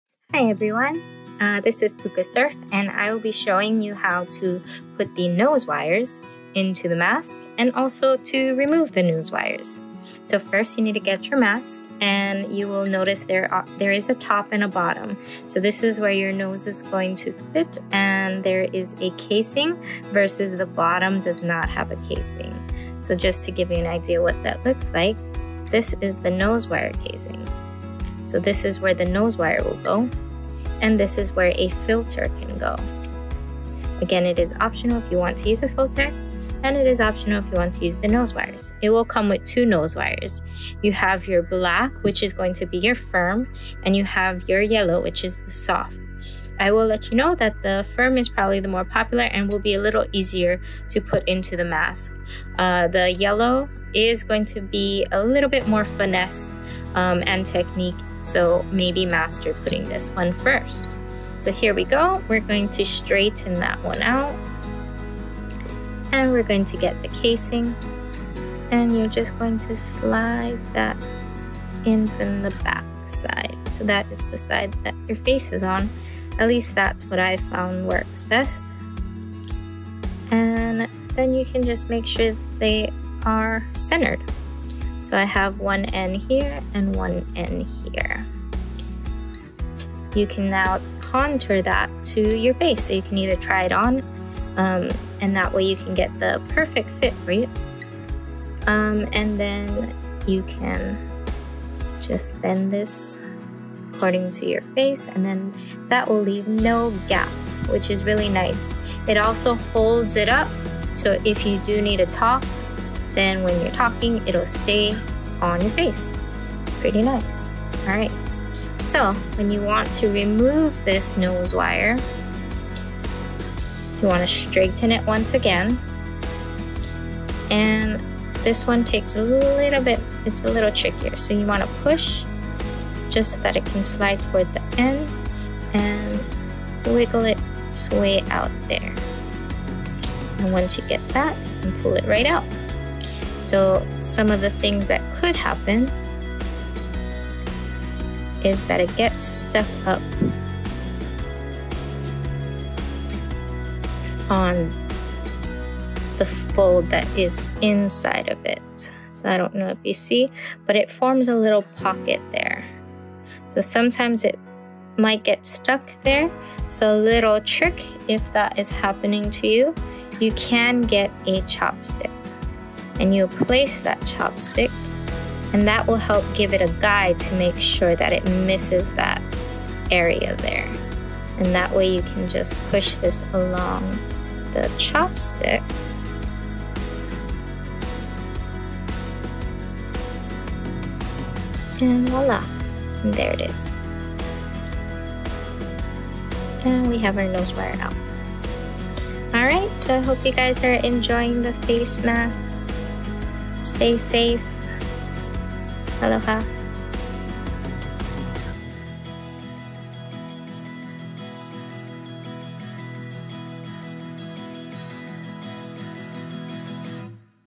Face Mask Tutorial : How to insert & remove nose wire
Puka Surf Co. non-medical cloth face masks come with one soft and one firm nose wire.  This video is a tutorial showing you how to identify the nose casing and how to properly insert the wire for a comfortable fit and how to remove it before washing.

Stay Stoked my fellow humans.